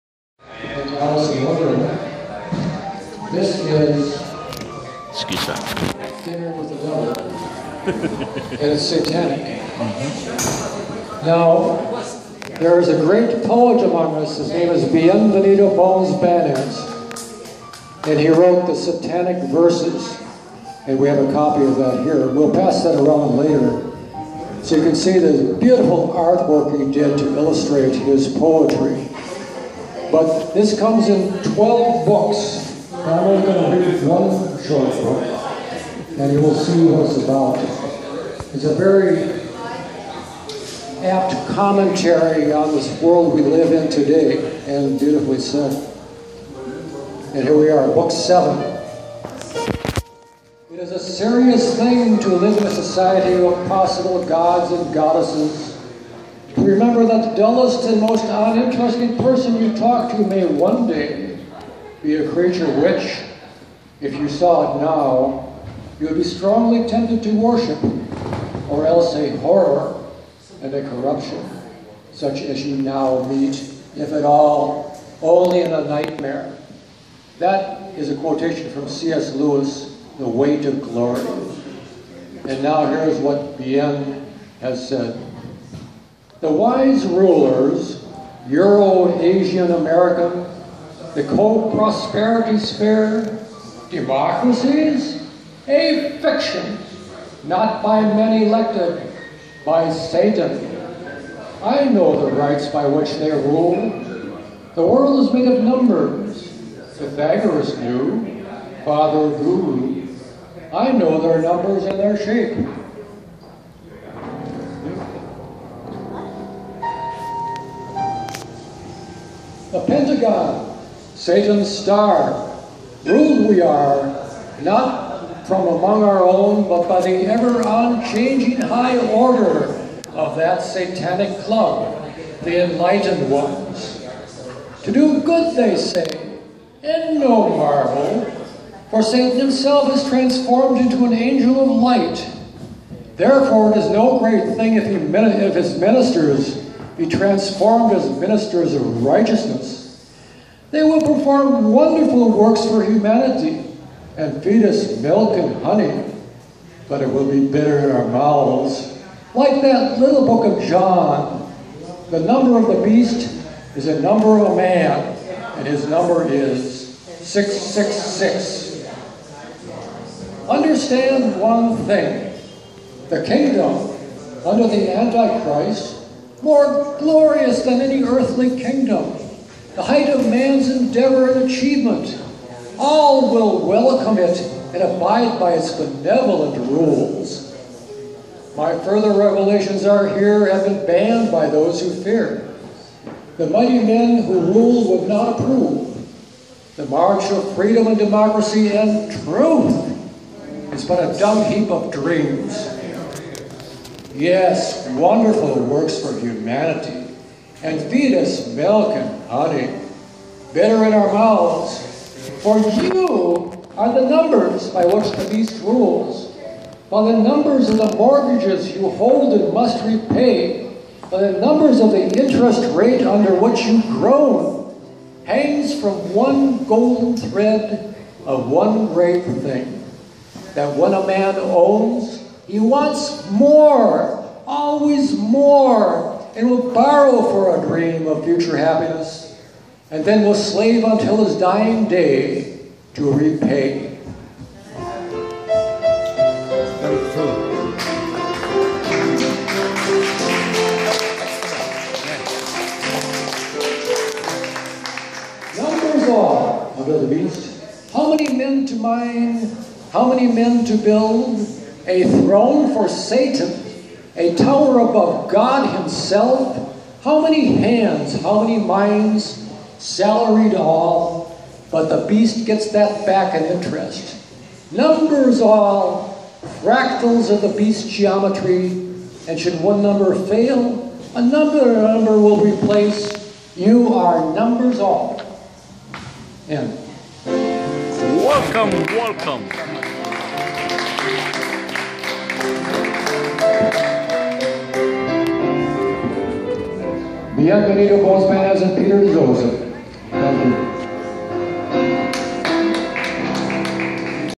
Terrance Lindall read Satanic Verses of Bones Banez - Theos-666 Surreal-blasphemous Art World!
HORROR SHOW 2016 Terrance Lindall reads the SATANIC VERSES of Bones Banez Book of the Damned
Book Seven "The Satanic Verses of Bones Banez." As Revealed to Terrance Lindall/ It is a fate that sweeps like a storm in the night. It is the Jungian collective unconscious of our species that has simmered as a dormant volcano of the multitudes and explodes periodically with a force that can astound and shake humanity. We awaken from our humdrum every day existence to a world of horror and find another more intense reality behind the superficial mask of everyday life…in our nightmares or captured by our artists for the public to share! Halloween is perhaps one of the oldest holidays on record, dating back more than 2,000 years to the days of the Celts and their high priests, the Druids. While associated with Ireland, the Celtic people lived across a wide region that included Great Britain and northern France, and marked their new year on November 1st. This was the end of summer and a time of harvest, as the Celts began to prepare for the cold, dark winter. For many, this time of year was marked by a period of death. For a moment in October at the WAH Center we can all escape from the accepted ways of behaving – toppling our obedience to the commonplace, our slavery to doing meekly what does not offend others, the “go along, get along” mentality.